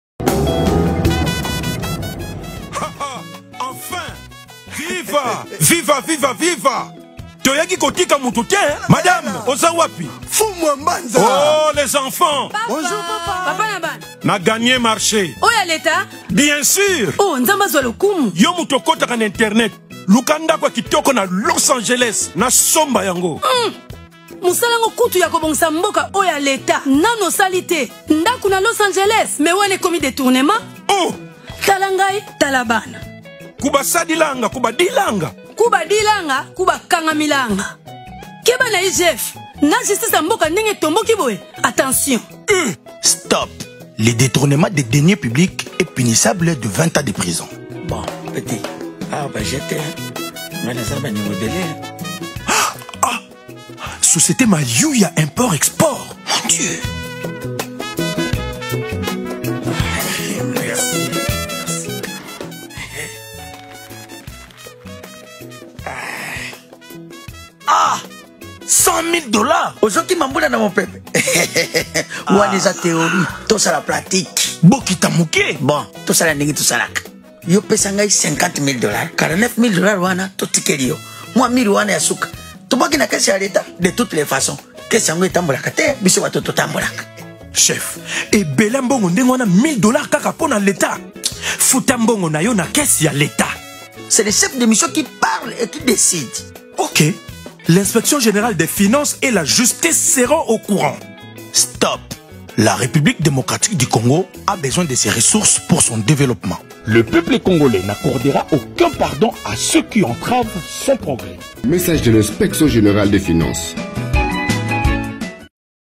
(vision du PDT Fatshi): Fatshi lutte contre la corruption en RDC par le changement de mentalité
campagne contre la corruption  en RDC